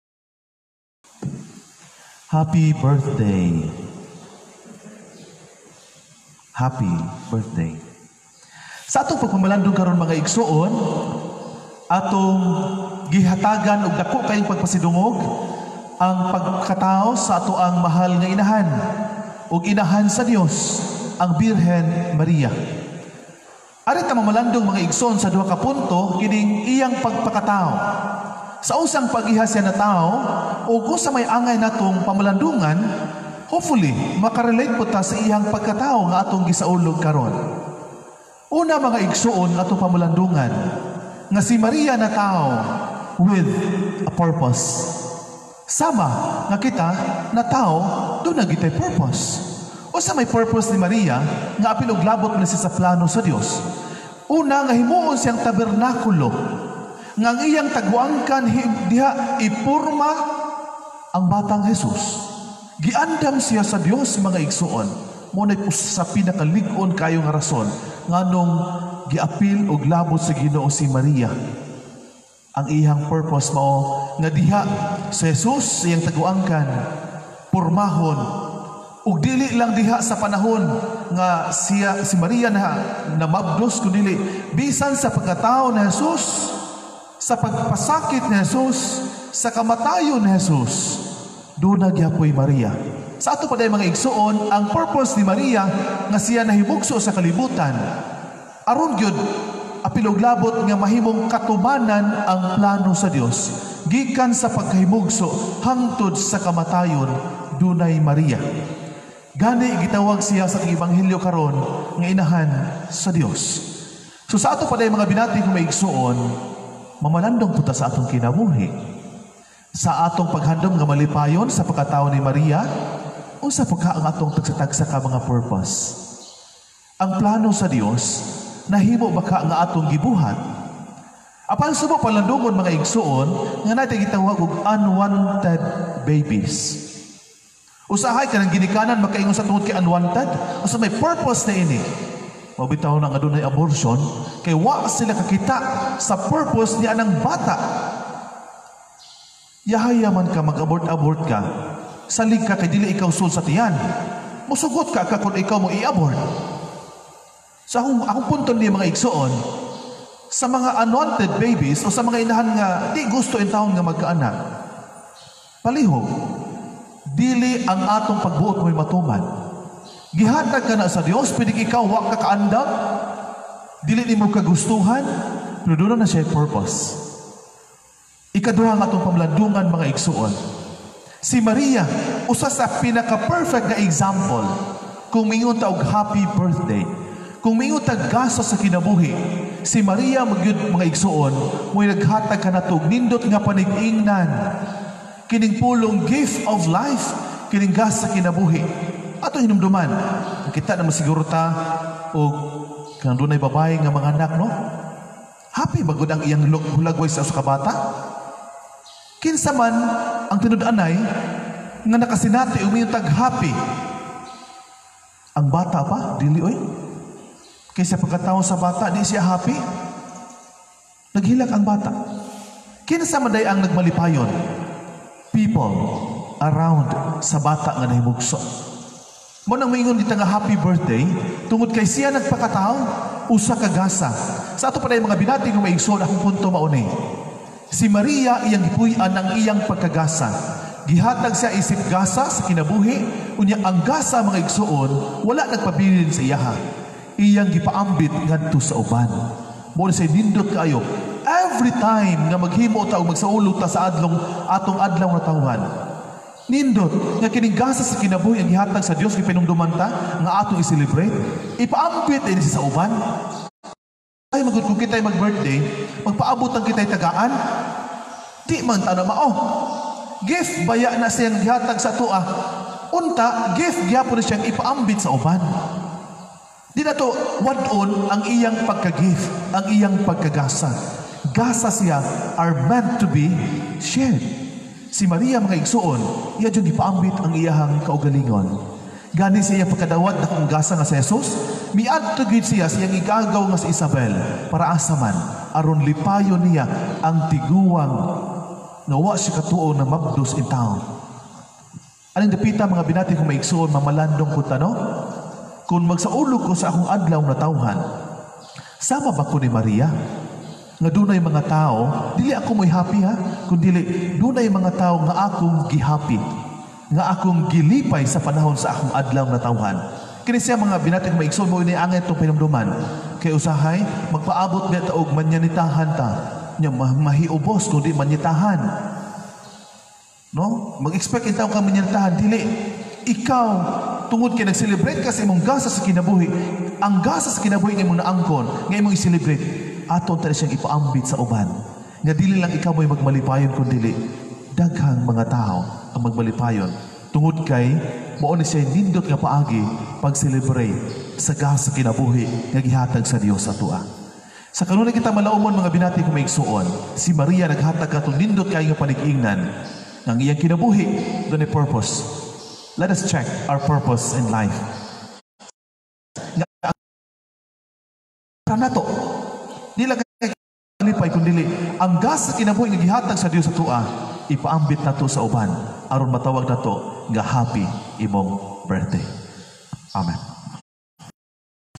PAMALANDONG SA PARING BOL-ANON: NGANONG GIPAKATAWO SI MARIA (HOMILY SA BIRTHDAY OF MARY; SEPT. 8)
Homily on the birthday of Mama Mary, September 8.